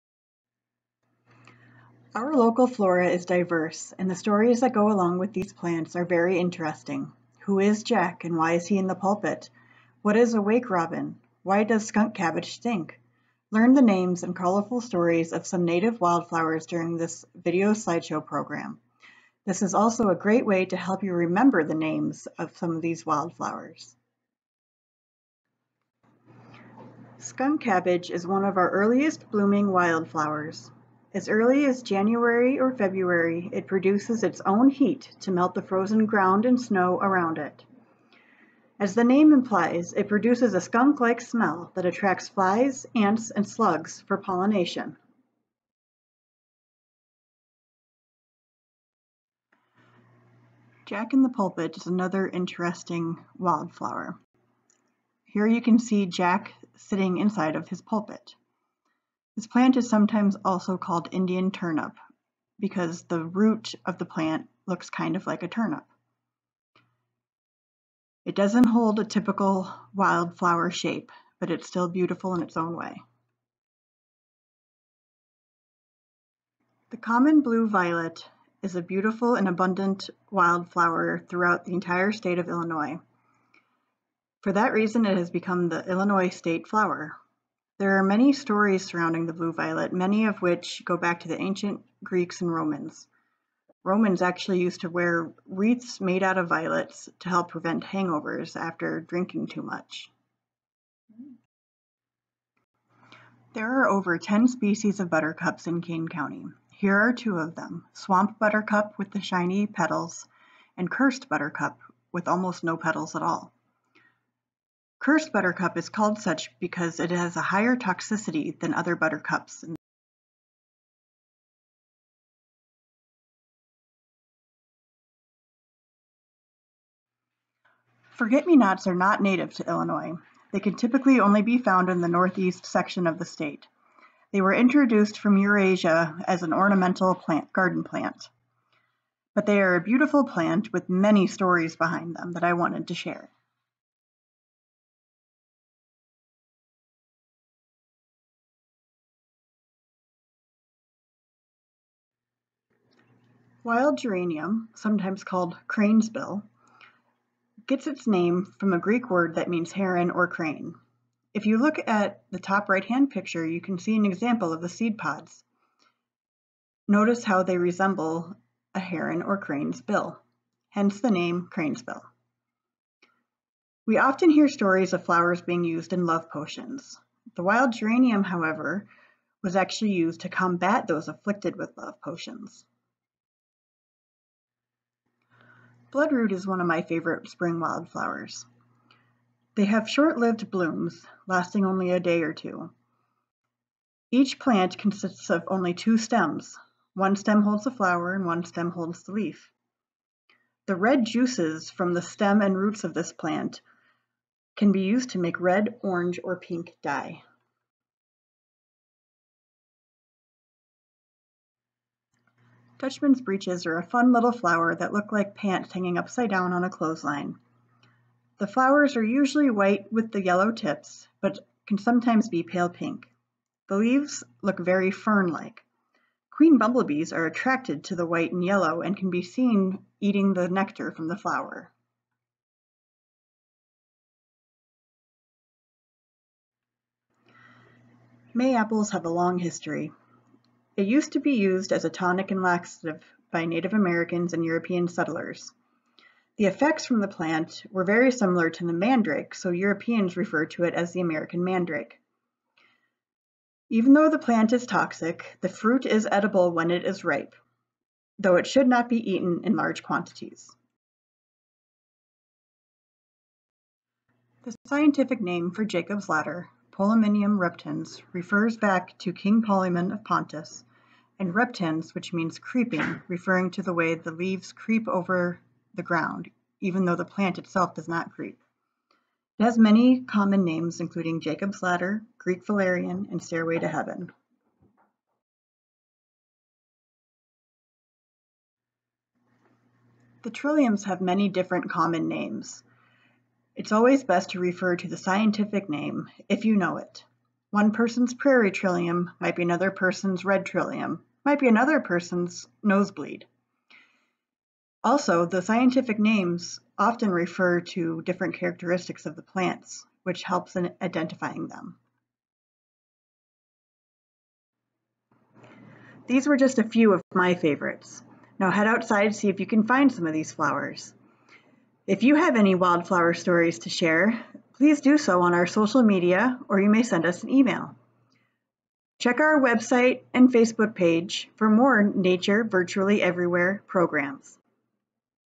Wildflower Folklore - Video Slideshow Program
Kane County's local flora is diverse. The stories that go along with these plants are very interesting! Who is Jack and why is he in the pulpit? What is a "Wake Robin?" Why does Skunk Cabbage stink? Learn the names and colorful stories of some native wildflowers during this video slideshow nature program, narrated by Naturalist Erica. This series is part of our Nature Virtually Everywhere program series.